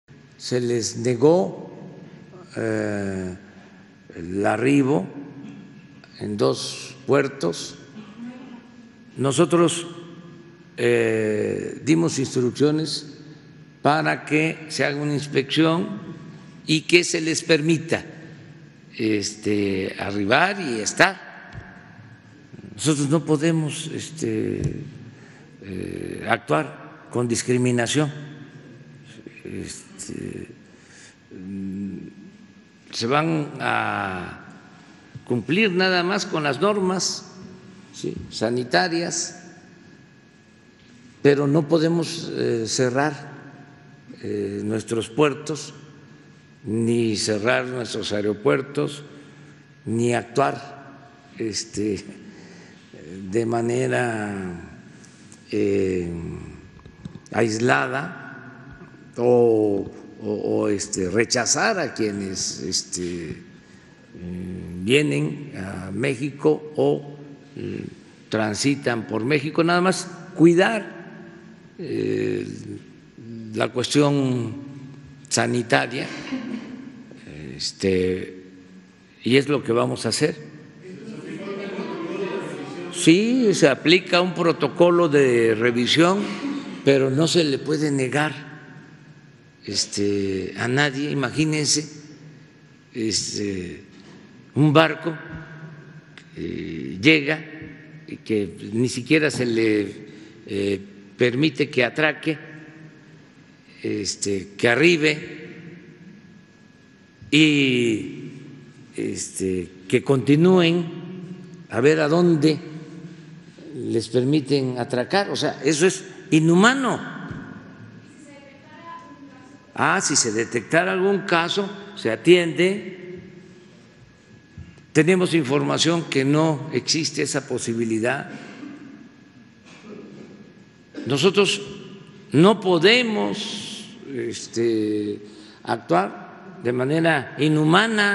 Respecto al crucero, no actuaremos con discriminación: AMLO
El presidente López Obrador comentó respecto al crucero Meraviglia «dimos instrucciones para que arribe a nuestro puerto, no actuaremos con discriminación y se cumplirá con las inspecciones y normas sanitarias».